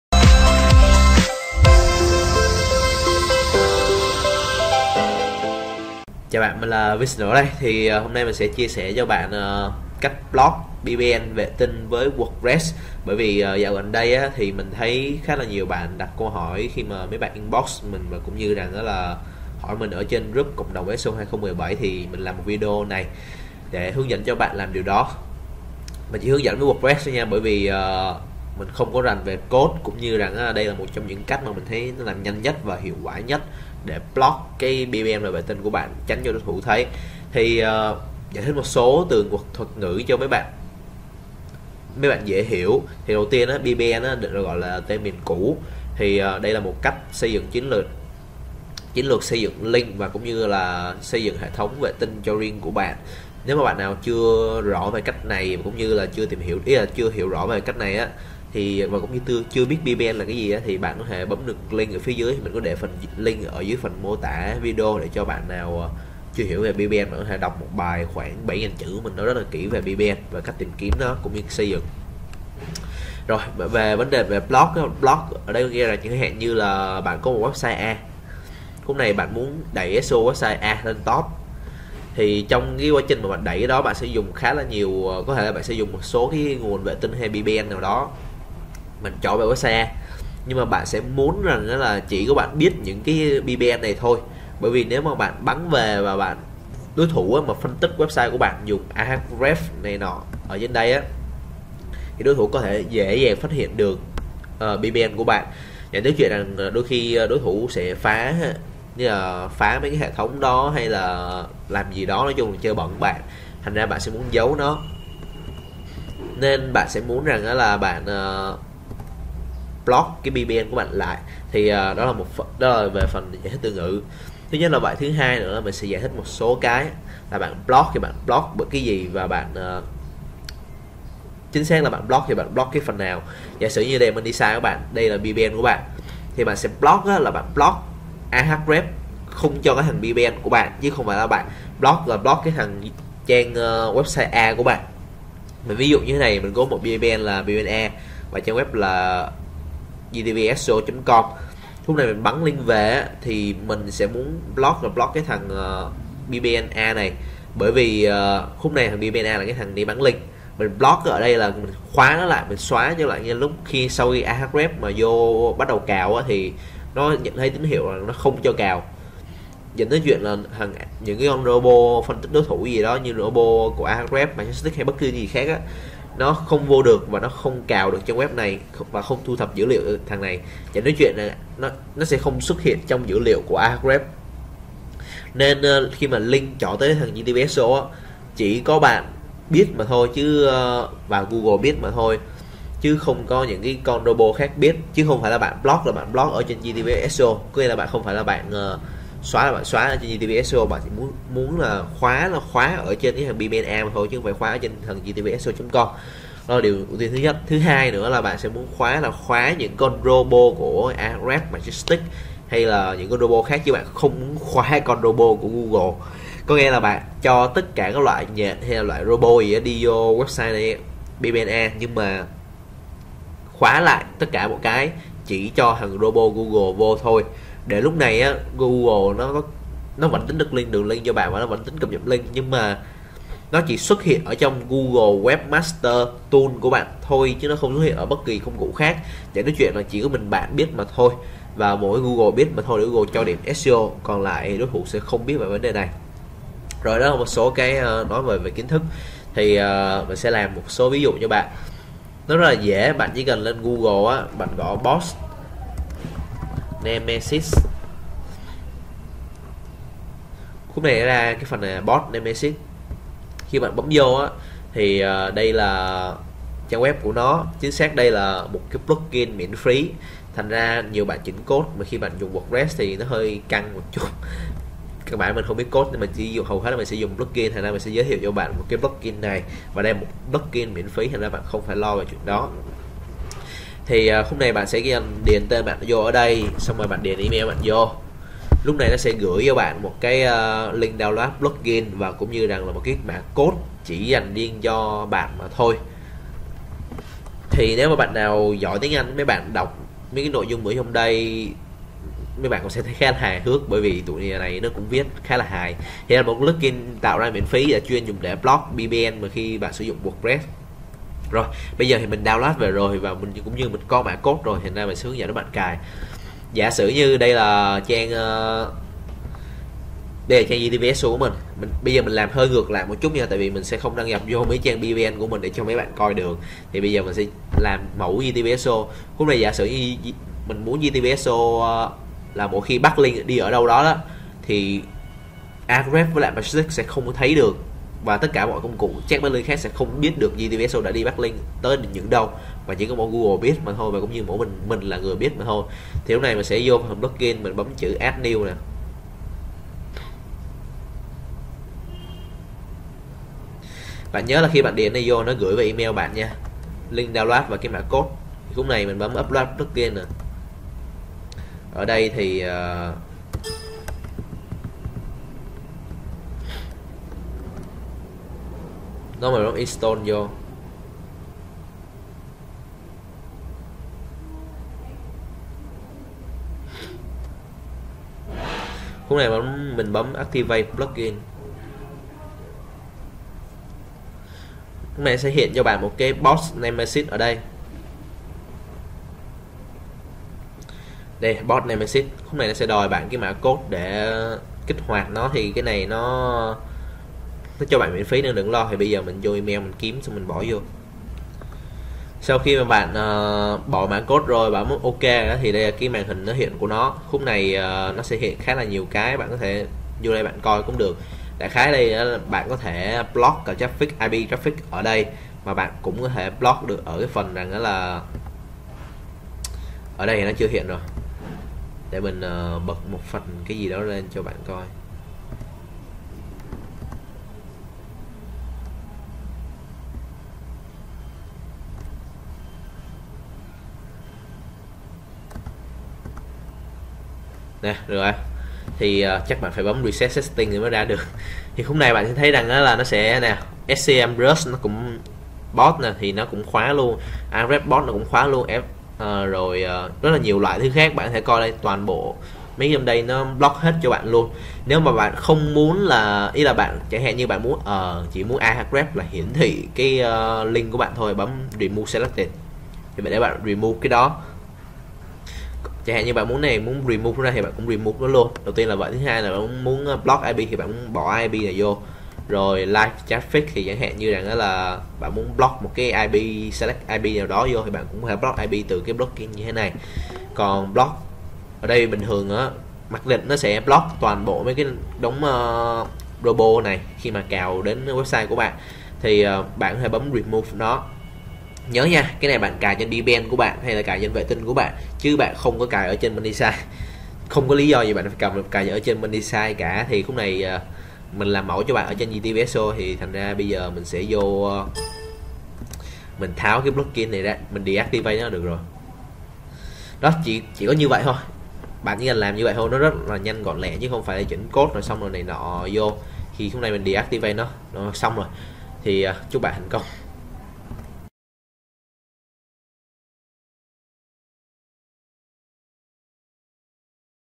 CÁCH BLOCK PBN/VỆ TINH KHỎI ĐỐI THỦ VỚI WORDPRESS - Học SEO 60
Block PBN/vệ tinh là gì? Tại sao phải block PBN/vệ tinh khỏi đối thủ? Các lưu ý khi block PBN và hướng dẫn cách làm A-z đều có trong video này.